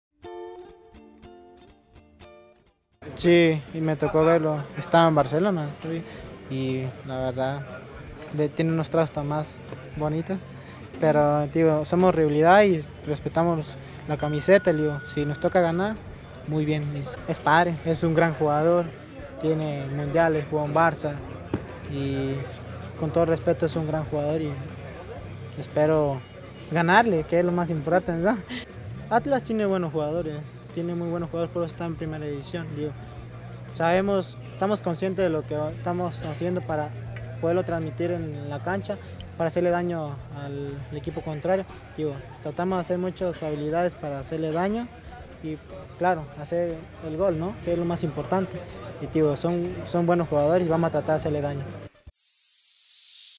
Orbelín veía a Márquez en tele, ahora quiere hacerle dañ
Por su corta edad, Orbelín Pineda tuvo la posibilidad de ver a Rafael Márquez en su mejor nivel, desplegando su futbol en el FC Barcelona, club en el cual se ganó el reconocimiento mundial. Ahora,...